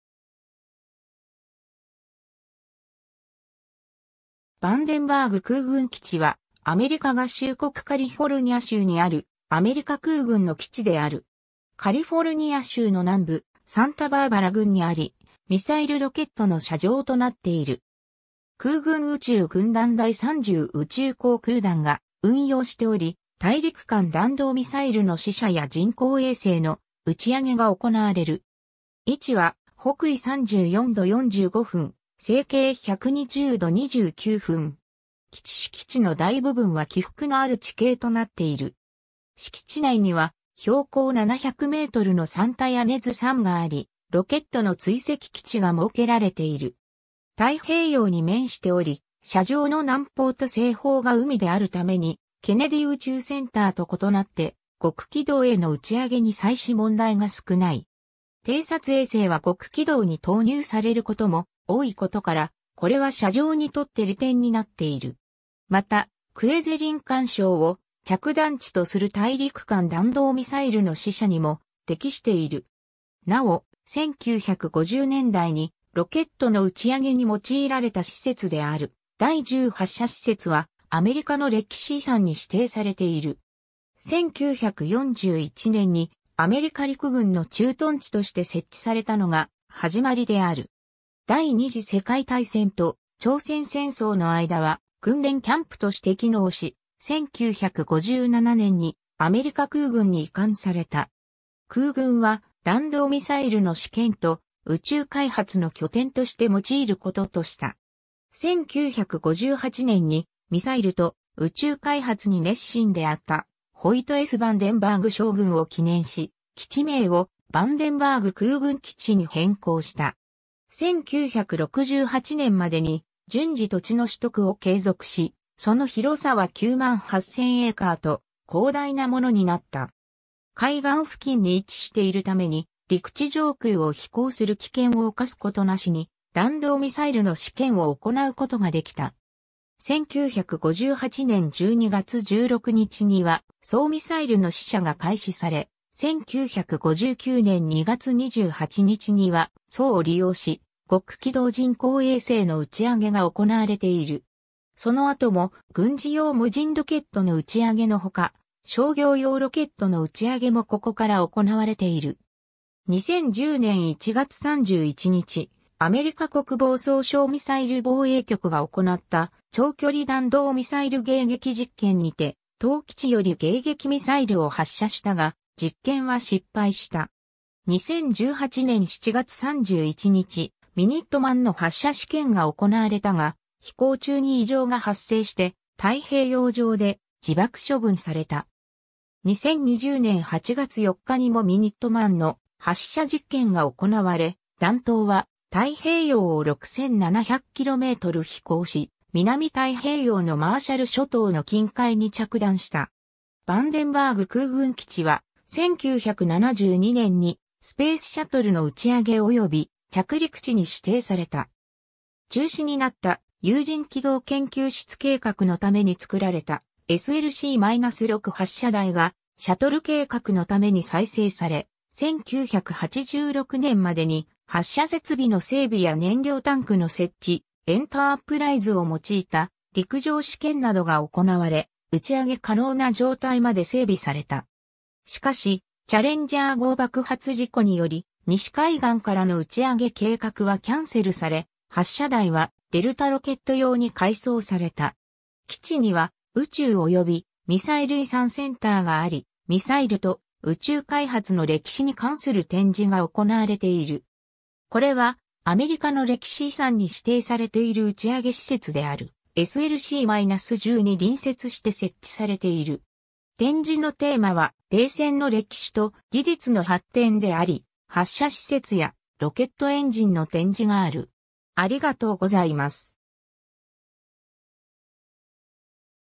ヴァンデンバーグ空軍基地
ヴァンデンバーグ空軍基地（ヴァンデンバーグくうぐんきち、Vandenberg Air Force Base）は、アメリカ合衆国カリフォルニア州にあるアメリカ空軍の基地である。
カリフォルニア州の南部、サンタバーバラ郡にあり、ミサイル・ロケットの射場となっている。
 空軍宇宙軍団第30宇宙航空団が運用しており、大陸間弾道ミサイルの試射や人工衛星の打ち上げが行われる。
位置は、北緯34度45分、西経120度29分。
基地敷地の大部分は起伏のある地形となっている。
敷地内には標高700mのサンタヤネズ山があり、ロケットの追跡基地が設けられている。
 太平洋に面しており、射場の南方と西方が海であるために、ケネディ宇宙センターと異なって、極軌道への打ち上げに際し問題が少ない。
偵察衛星は極軌道に投入されることも多いことから、これは射場にとって利点になっている。
また、クェゼリン環礁を着弾地とする大陸間弾道ミサイルの試射にも適している。
 なお、1950年代にロケットの打ち上げに用いられた施設である、第10発射施設 (Space Launch Complex 10 ; SLC-10) はアメリカの歴史遺産に指定されている。
 1941年にアメリカ陸軍の駐屯地（キャンプ・クーク Camp Cooke）として設置されたのが始まりである。
第二次世界大戦と朝鮮戦争の間は、訓練キャンプとして機能し、1957年にアメリカ空軍に移管された。
空軍は、弾道ミサイルの試験と宇宙開発の拠点として用いることとした。
1958年にミサイルと宇宙開発に熱心であったホイト・S・ヴァンデンバーグ将軍（アメリカ空軍・第2代参謀長）を記念し、基地名をヴァンデンバーグ空軍基地に変更した。
 1968年までに、順次土地の取得を継続し、その広さは98,000エーカーと広大なものになった。
海岸付近に位置しているために、陸地上空を飛行する危険を侵すことなしに、弾道ミサイルの試験を行うことができた。
 1958年12月16日には、ソー・ミサイルの試射が開始され、1959年2月28日には、ソーを利用し極軌道人工衛星の打ち上げが行われている。
 その後も、軍事用無人ロケットの打ち上げの他、商業用ロケットの打ち上げもここから行われている。
 2010年1月31日、アメリカ国防総省ミサイル防衛局が行った長距離弾道ミサイル迎撃実験にて、当基地より迎撃ミサイルを発射したが、実験は失敗した。
 2018年7月31日、ミニットマンの発射試験が行われたが、飛行中に異常が発生して太平洋上で自爆処分された。
2020年8月4日にもミニットマンの発射実験が行われ、弾頭は太平洋を6700キロメートル飛行し南太平洋のマーシャル諸島の近海に着弾した。
 ヴァンデンバーグ空軍基地は、1972年にスペースシャトルの打ち上げおよび着陸地に指定された。
中止になった有人軌道研究室計画のために作られたSLC-6発射台がシャトル計画のために再生され、1986年までに発射設備の整備や燃料タンクの設置、エンタープライズを用いた陸上試験などが行われ、打ち上げ可能な状態まで整備された。
しかし、チャレンジャー号爆発事故により、西海岸からの打ち上げ計画はキャンセルされ、発射台はデルタロケット用に改装された。
 基地には、宇宙及びミサイル遺産センター（Space and Missile Heritage Center）があり、ミサイルと宇宙開発の歴史に関する展示が行われている。
これは、アメリカの歴史遺産に指定されている打ち上げ施設である、SLC-10に隣接して設置されている。
展示のテーマは「冷戦の歴史」と「技術の発展」であり、発射施設やロケットエンジンの展示がある。